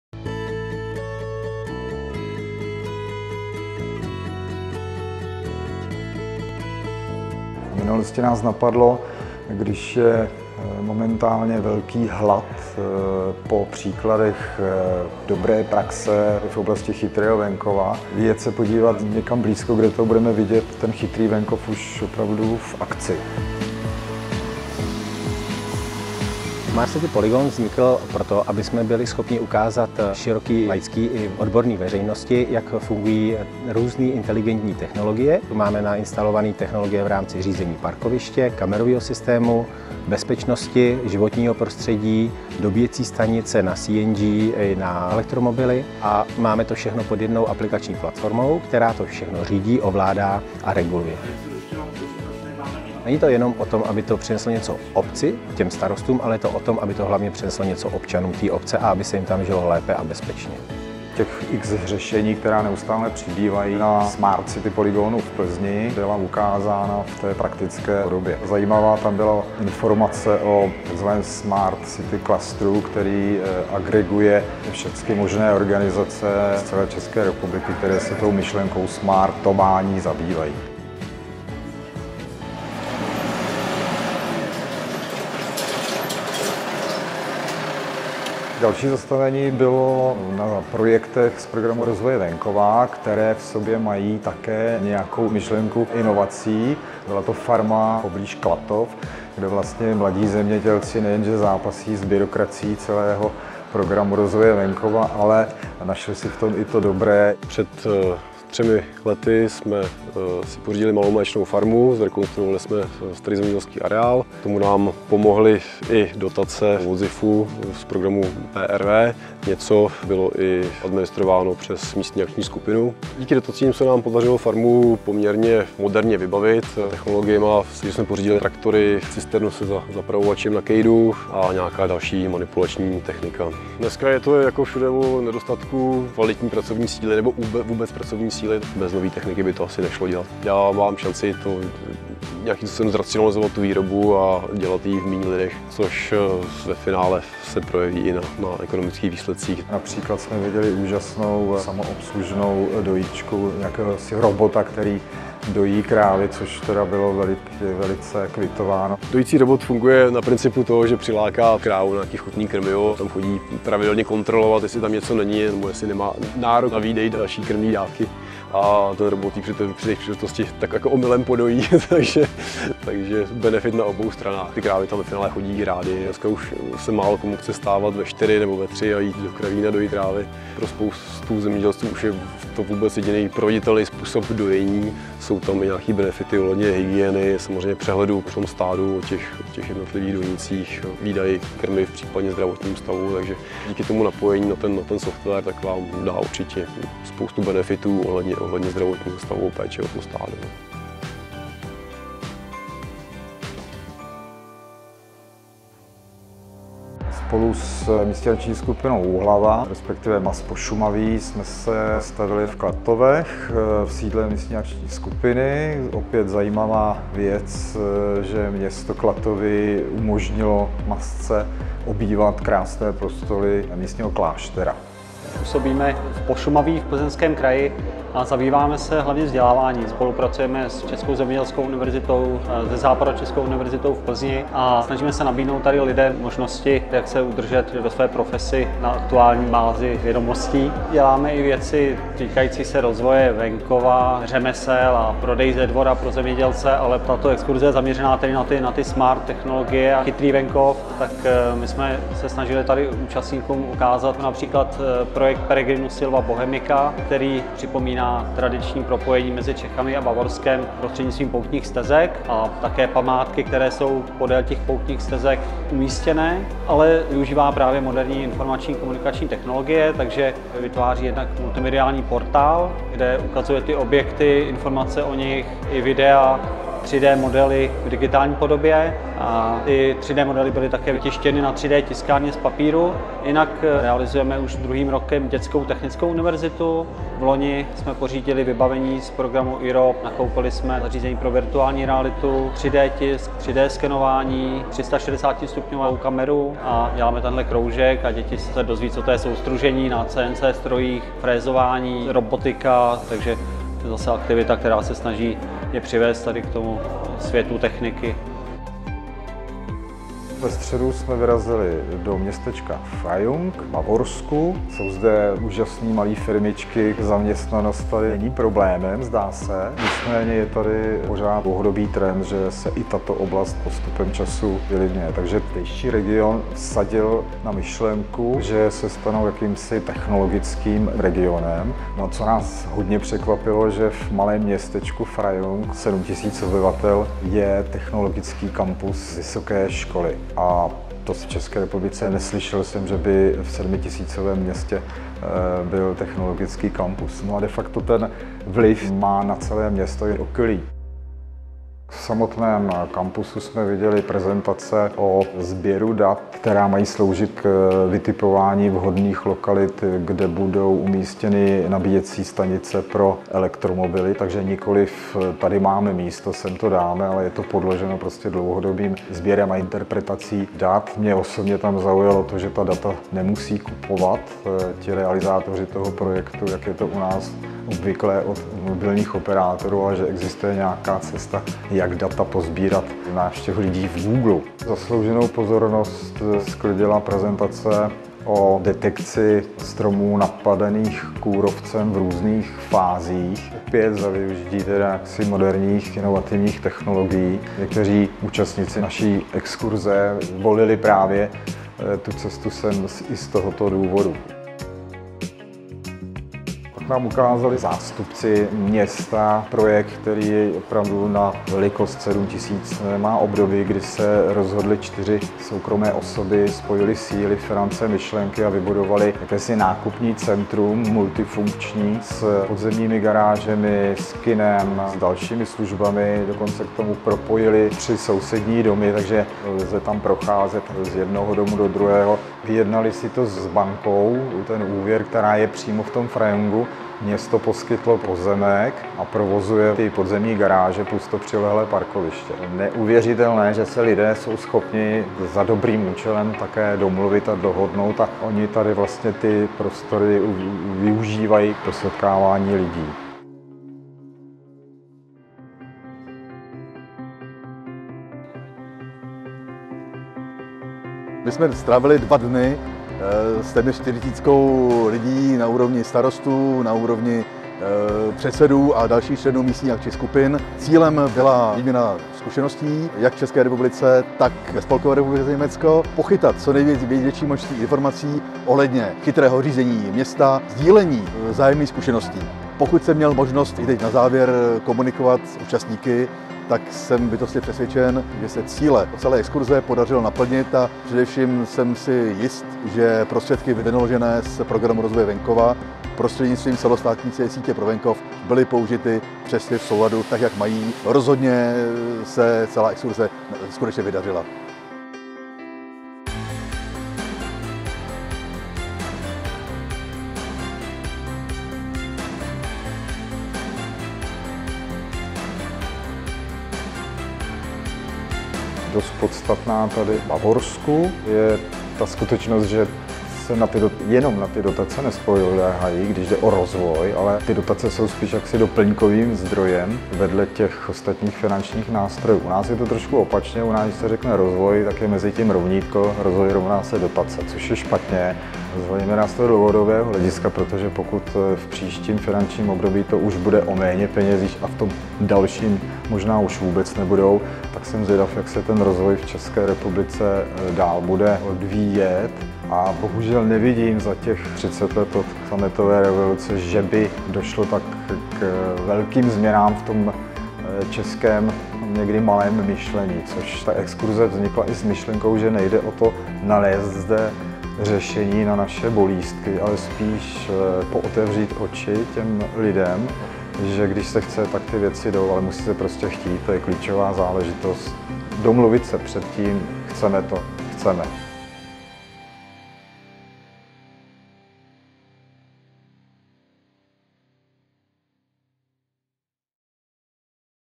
EXKURZE CHYTRÝ VENKOV
Kombinovaná tuzemsko-zahraniční exkurze byla organizována za účelem načerpání
příkladů dobré praxe ve venkovských oblastech s aspektem na koncept Smart Village.

Exkurze CHYTRÝ VENKOV proběhla ve dnech 24. – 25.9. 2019
v Plzeňském kraji a na území Bavorska v Německu.